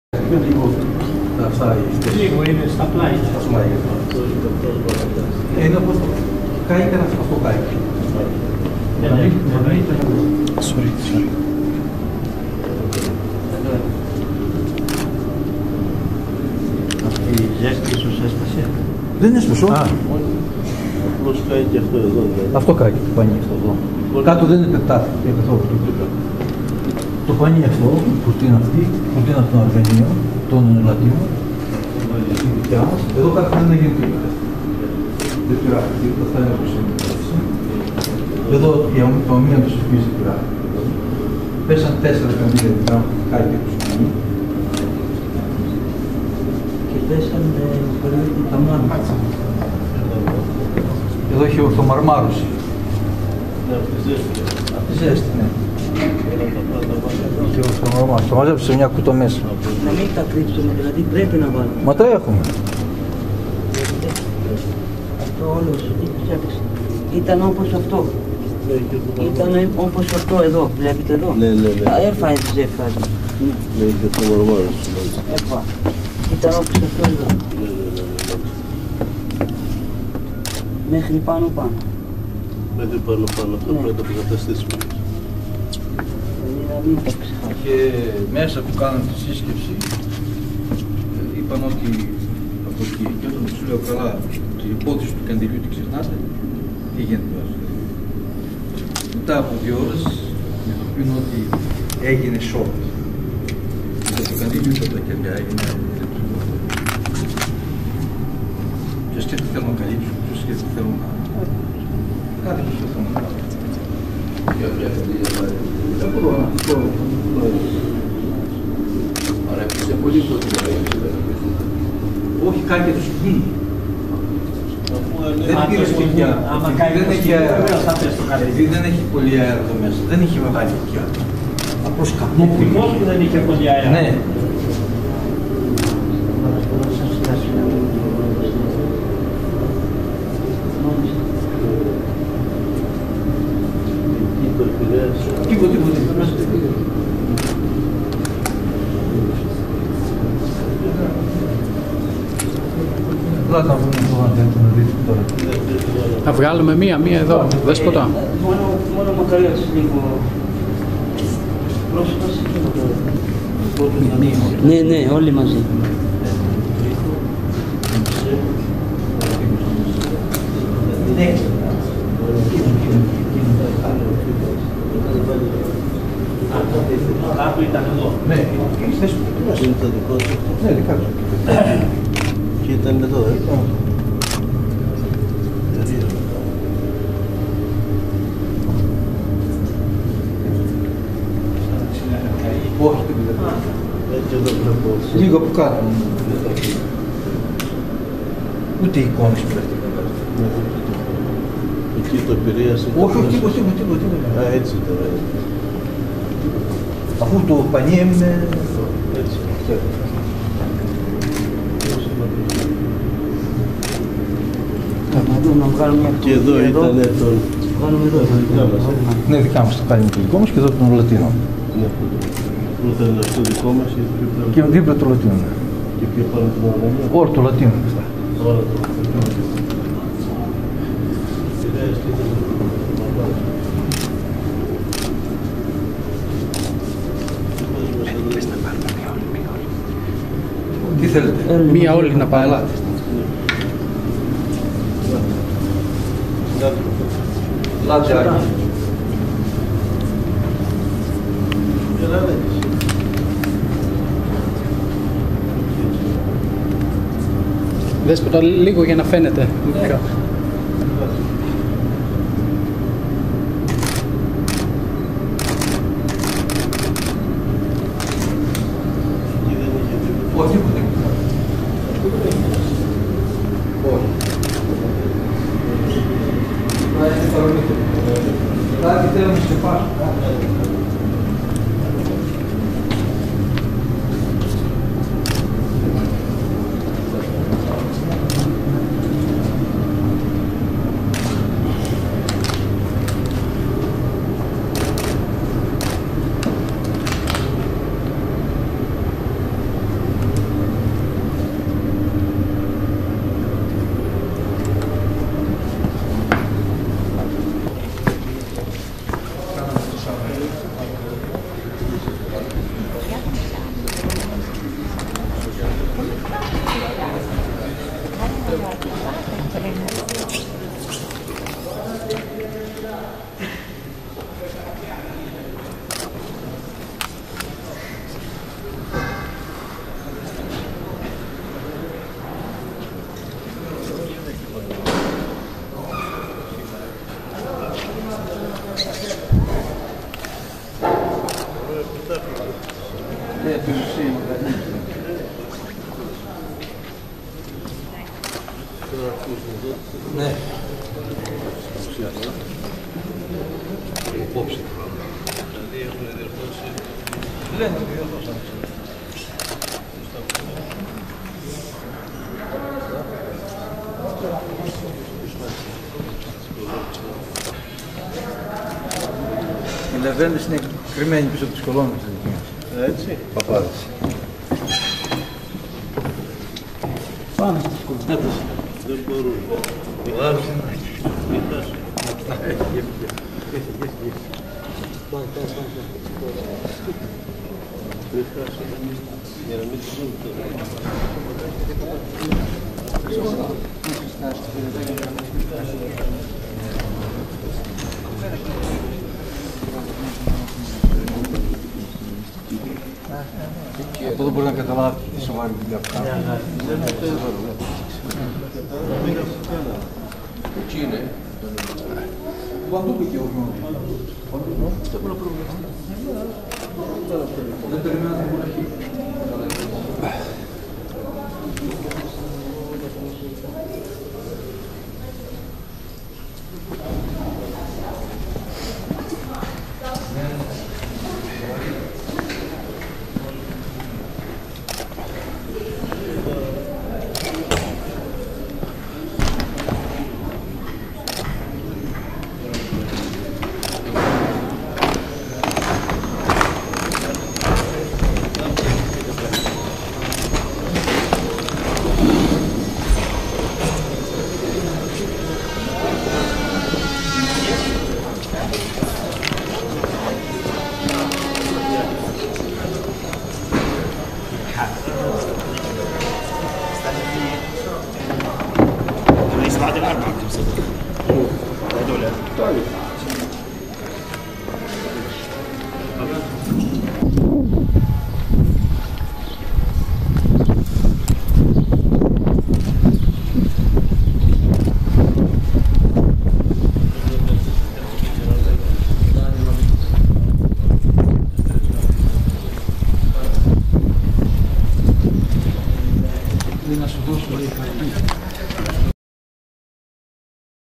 ΠΥΡΚΑΪΑ ΣΤΟ ΘΕΟΔΕΓΜΟΝ ΣΠΗΛΑΙΟ ΤΗΣ ΒΗΘΛΕΕΜ.
Ο Μακαριώτατος Πατριάρχης Ιεροσολύμων κ.κ. Θεόφιλος και η Συνοδεία Αυτού ενημερώνονται για την πυρκαϊα που σημειώθηκε το πρωί της Τρίτης 14/27 Μαίου στο Θεοδέγμον Σπήλαιο και για τις εργασίες που γίνονται στη Βασιλική της Γεννήσεως.